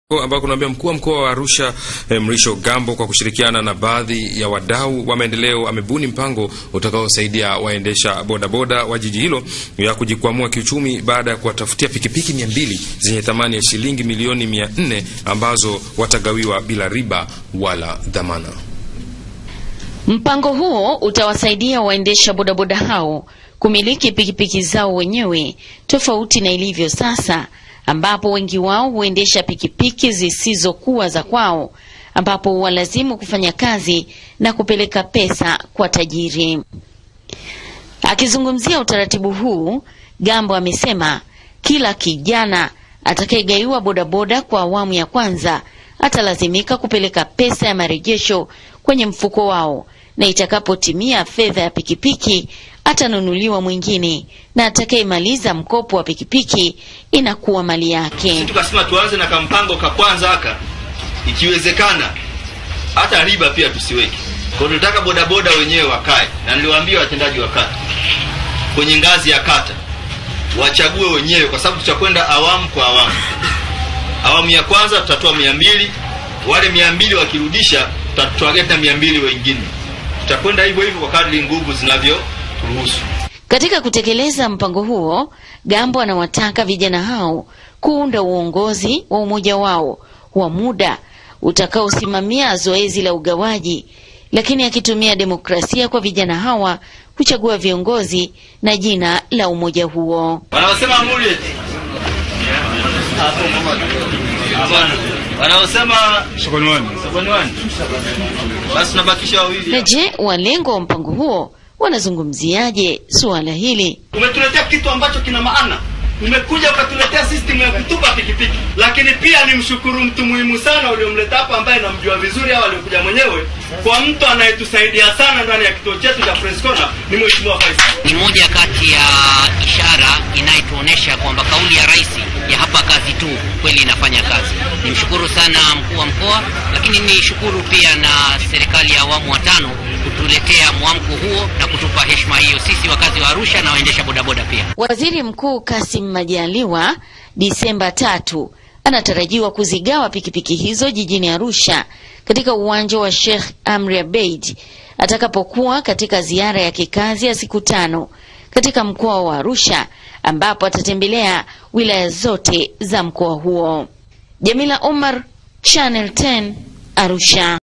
Wakopeshwa Pikipiki Bila Riba
Mkuu wa mkoa wa Arusha Mrisho Gambo kwa kushirikiana na wadau wa maendeleo amebuni mpango wa kuwakopesha pikipiki madereva bodaboda ili waweze kujikwamua kiuchumi.